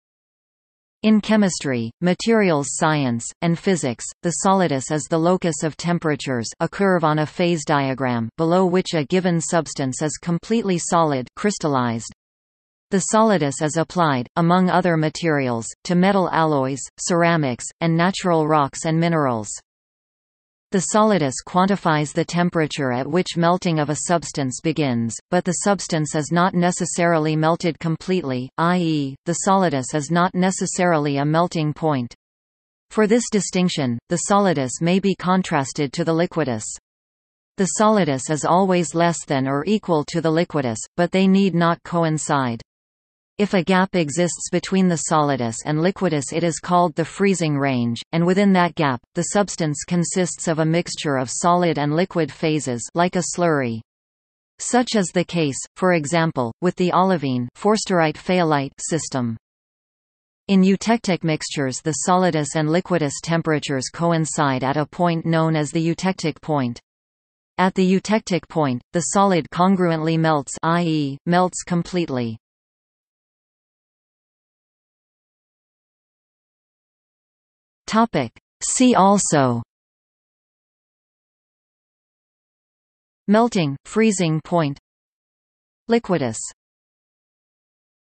Solidus (chemistry) | Wikipedia audio article
00:01:14 See also

- Socrates

SUMMARY
In chemistry, materials science, and physics, the solidus is the locus of temperatures (a curve on a phase diagram) below which a given substance is completely solid (crystallized). The solidus is applied, among other materials, to metal alloys, ceramics, and natural rocks and minerals.
The solidus quantifies the temperature at which melting of a substance begins, but the substance is not necessarily melted completely, i.e., the solidus is not necessarily a melting point. For this distinction, the solidus may be contrasted to the liquidus. The solidus is always less than or equal to the liquidus, but they need not coincide. If a gap exists between the solidus and liquidus it is called the freezing range, and within that gap, the substance consists of a mixture of solid and liquid phases (like a slurry). Such is the case, for example, with the olivine (forsterite-fayalite) system.
In eutectic mixtures the solidus and liquidus temperatures coincide at a point known as the eutectic point. At the eutectic point, the solid congruently melts (i.e., melts completely).